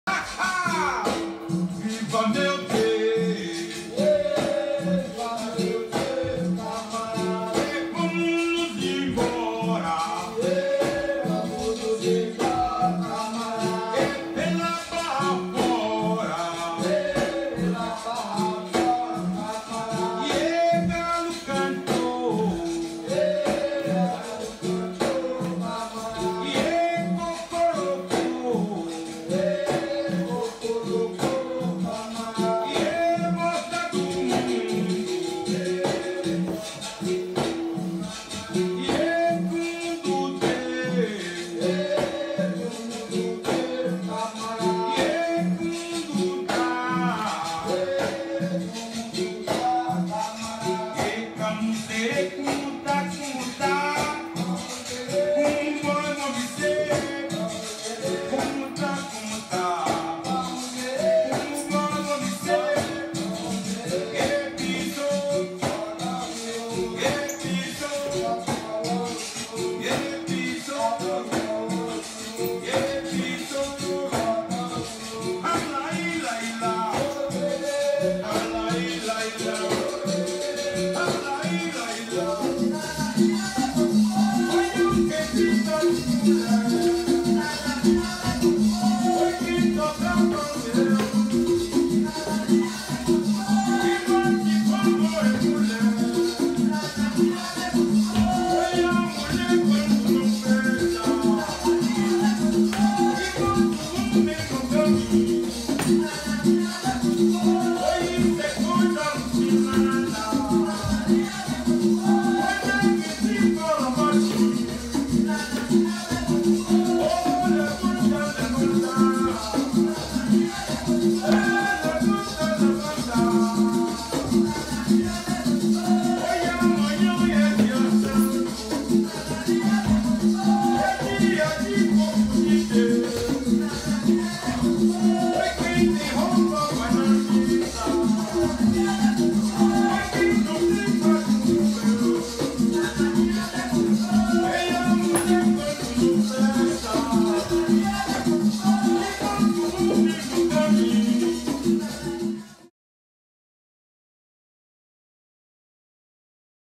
Capoeira da Bahia e do Brasil 🇧🇷 na veia 💪🏾
Lenilson capoeira retornando levemente aos treinos na China 🇨🇳